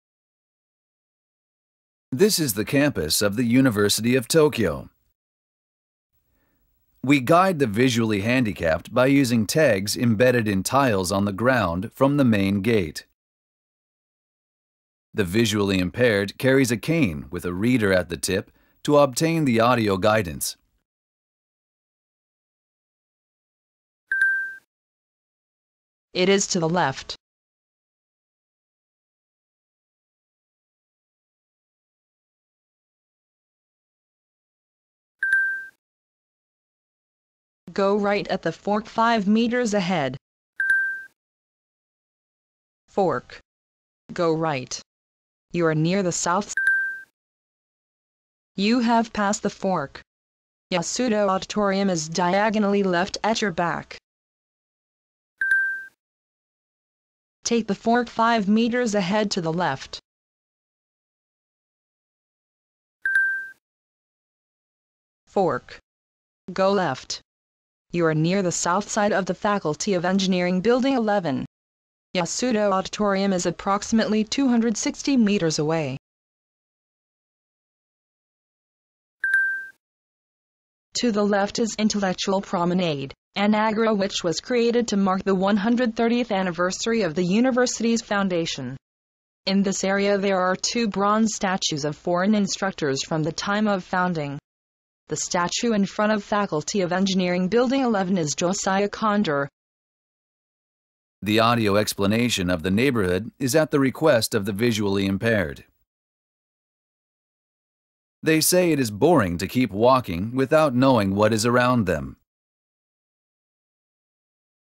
Blind Person Navigation
Navigation of Blind Person who uses  white cane with a RFID Reader